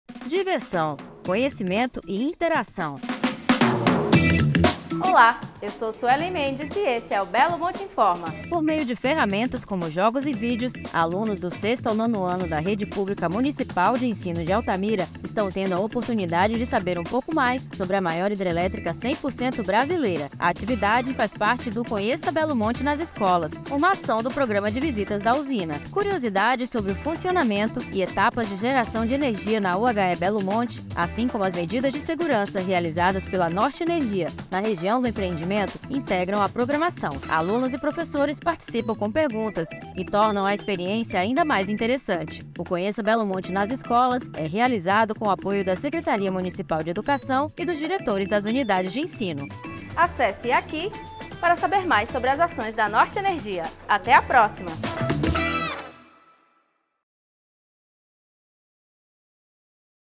Belo Monte Informa 31 - Conheça Belo Monte nas Escolas
O Belo Monte Informa, edição 31, mostra a atuação do Conheça Belo Monte nas Escolas, uma ação do Programa de Visitas da Usina. Voltada aos alunos do 6º ao 9º ano da rede pública municipal de Altamira, a atividade leva até o ambiente escolar informações sobre a geração de energia renovável e curiosidades sobre o funcionamento da maior usina hidrelétrica 100% brasileira.

O Conheça Belo Monte nas Escolas é realizado pela Norte Energia, em parceria com a Secretaria Municipal de Educação.